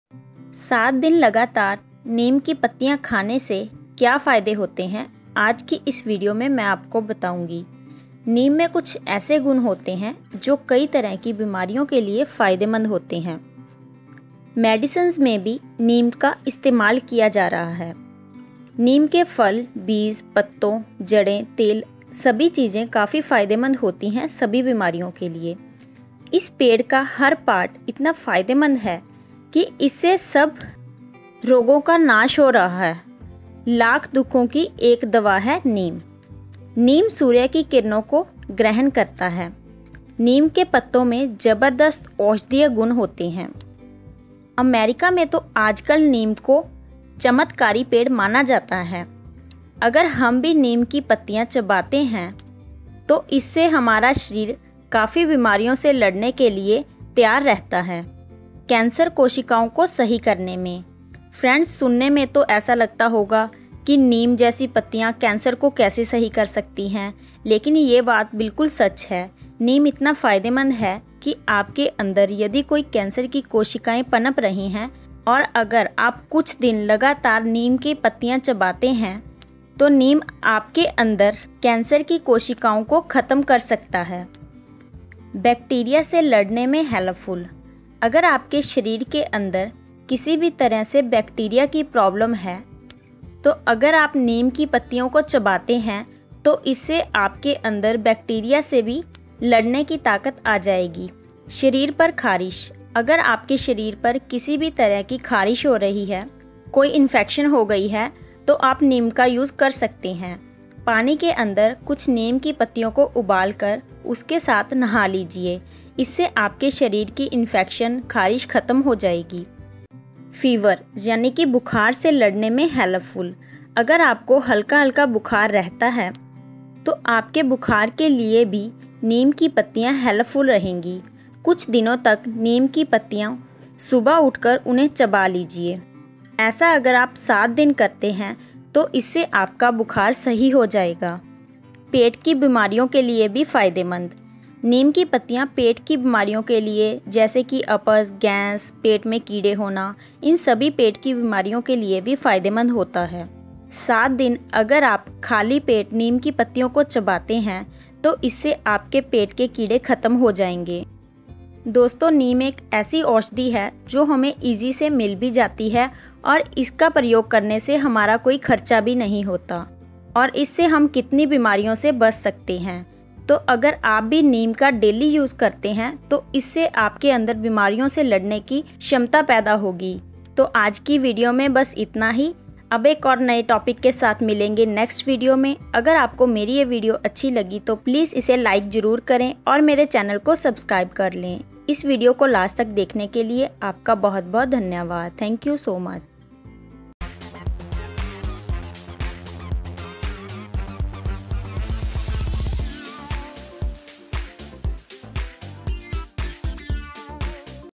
7 दिन लगातार नीम की पत्तिया खाने के बाद ऐसा हुआ जिसे सुनकर आप दंग रह जायेंगे || Beauty Experts
7 दिन लगातार नीम की पत्तिया खाने के बाद ऐसा हुआ जिसे सुनकर आप दंग रह जायेंगे || Beauty Experts ||
Thanks...